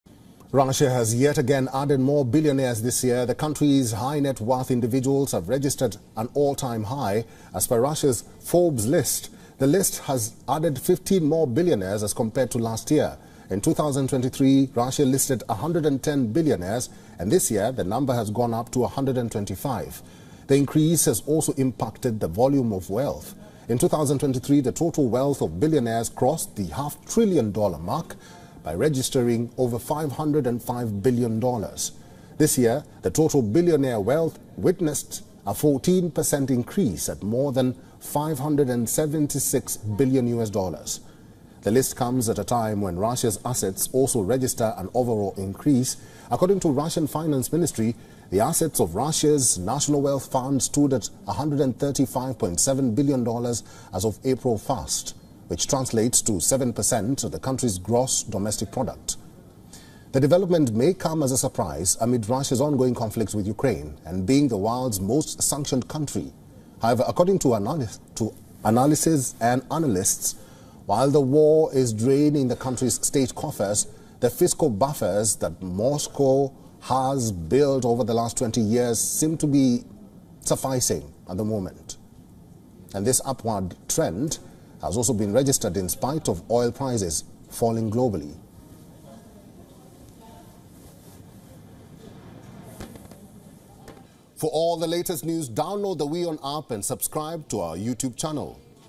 Forbes World's Billionaires List: Russia adds 15 more billionaires, up from 110 last year | WION
The total number of Russian billionaires climbed to 120 this year in Forbes World's Billionaires List, 15 more than last year and 37 more than in 2022. Watch in for more details!